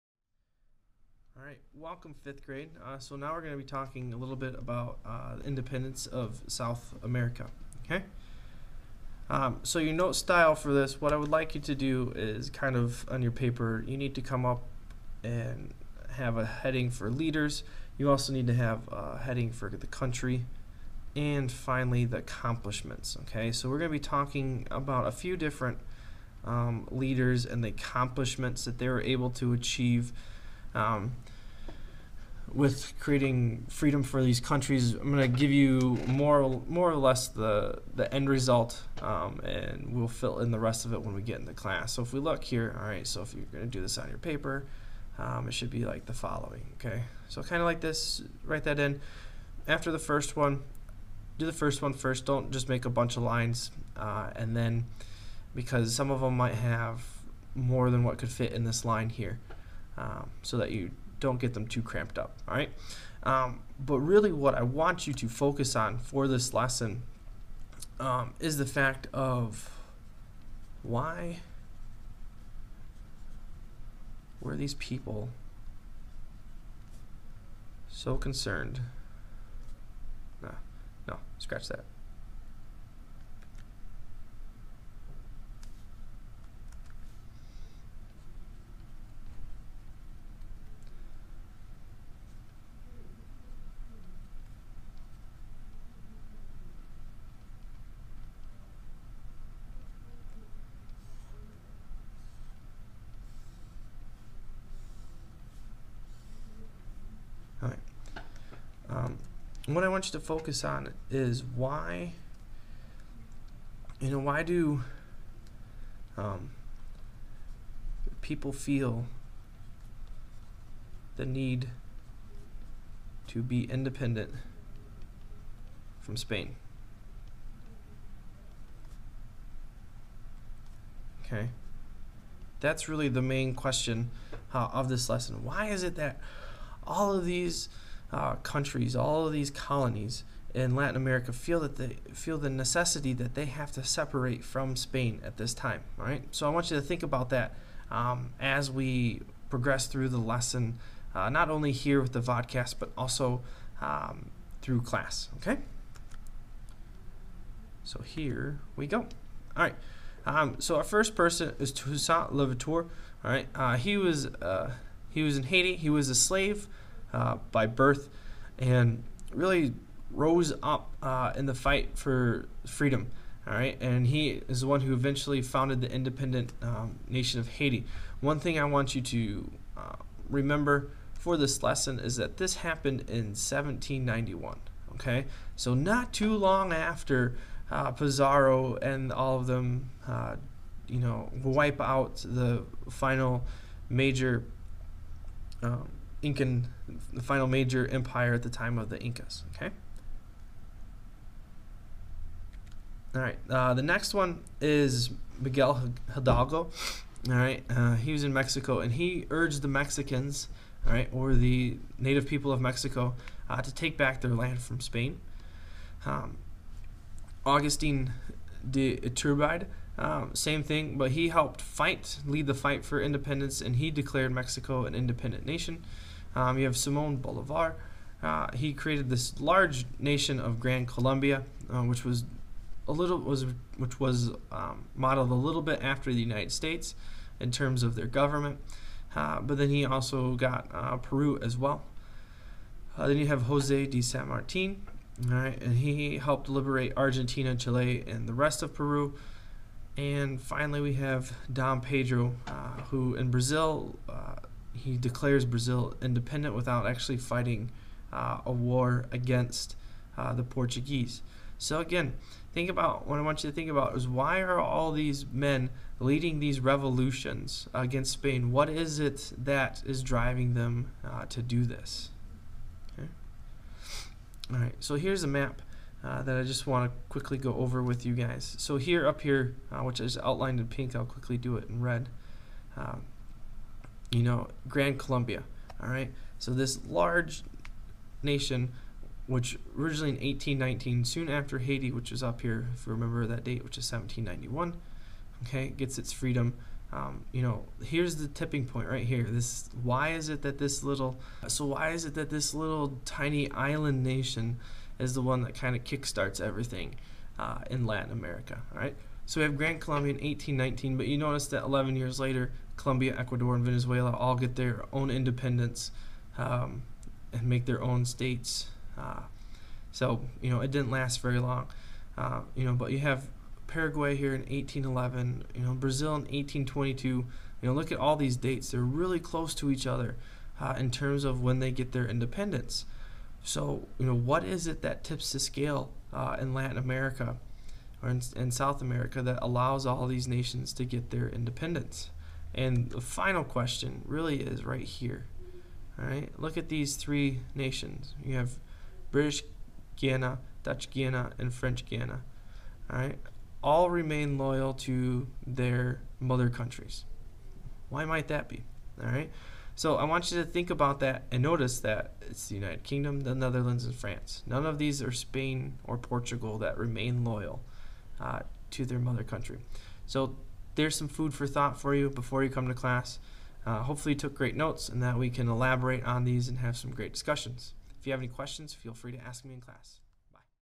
2.4 Independence in Latin America
A brief overview of the battles and fights for independence in Latin America